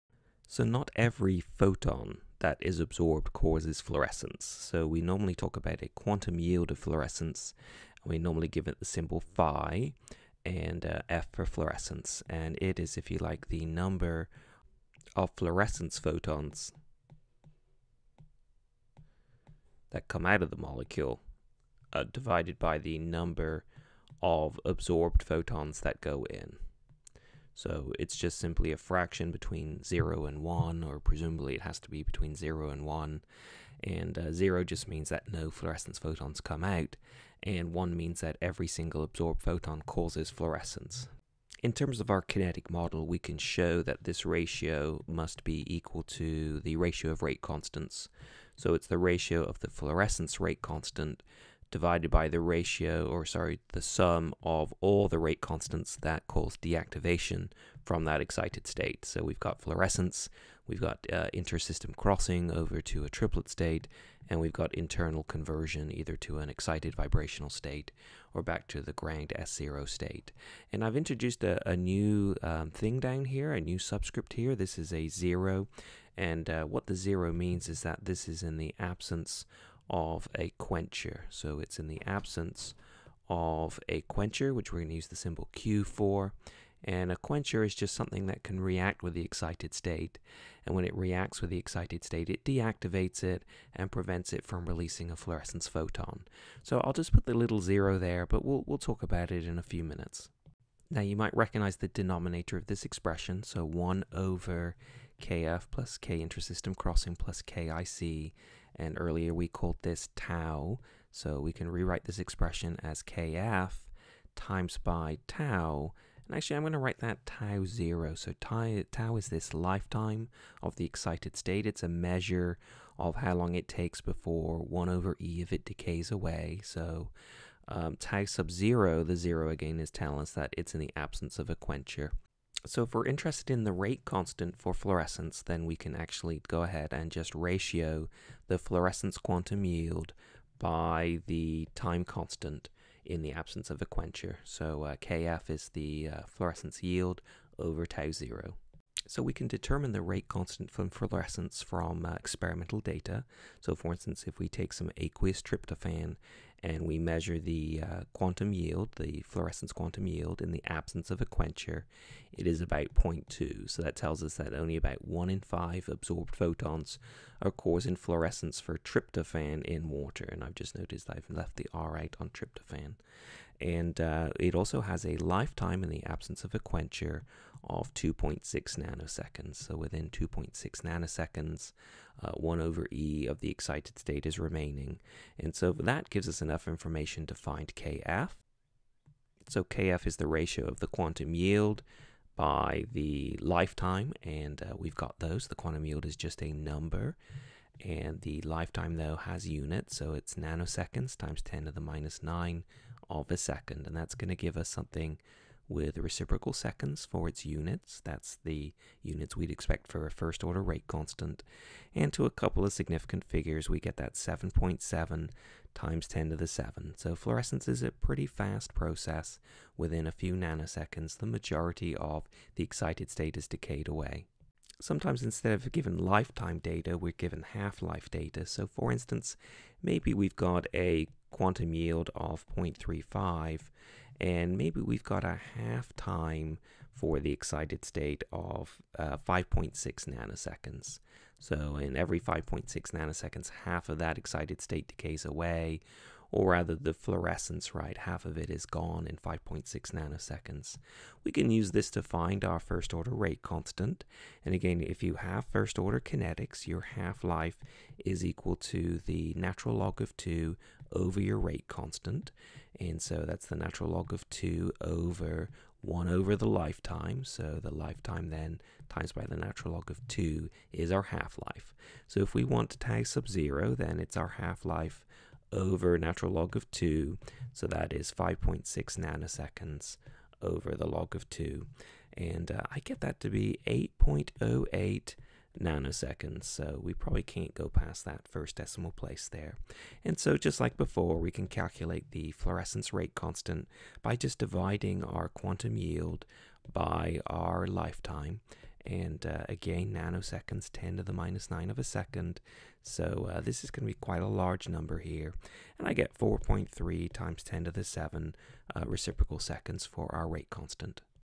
Quantum yield of fluorescence
We examine the quantum yield of fluorescence in more detail.
■ We see how it is related to the rate constant for fluorescence.
■ We also see how the relative ratio between the rate of fluorescence, and the rates of all other deactivating processes, is related to the quantum yield.